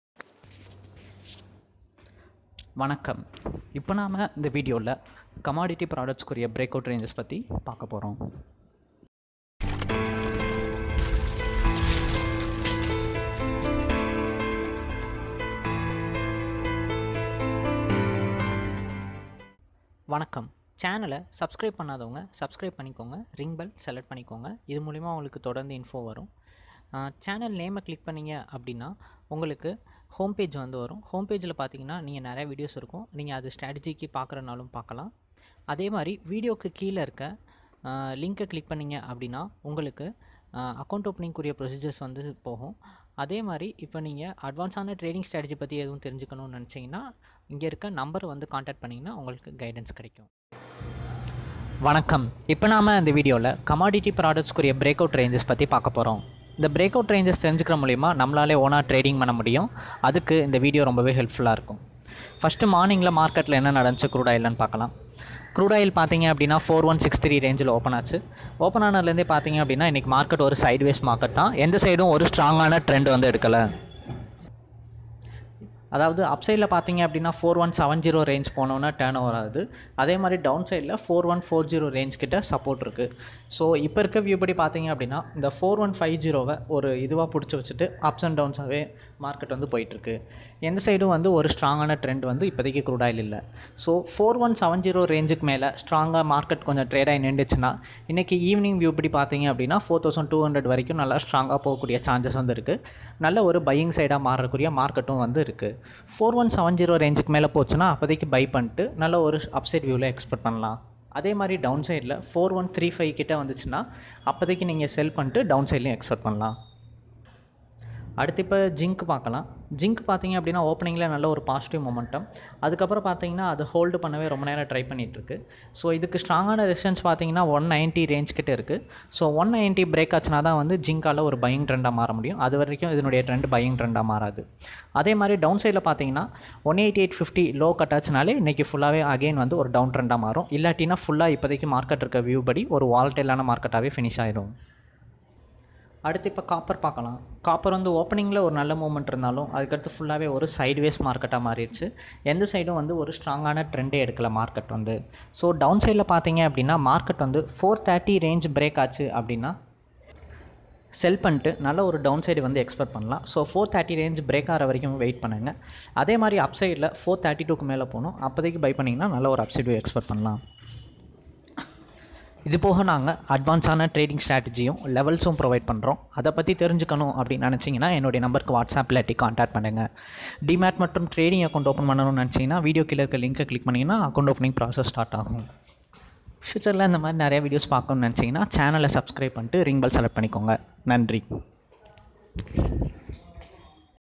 Commodity Live Levels | Crudeoil Evening Breakout Range 25 Nov
For Demat  account click Below Link - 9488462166

This video about Commodity Products like Crudeoil, cu & zinc

 Buying and selling range for Evening market free level!!!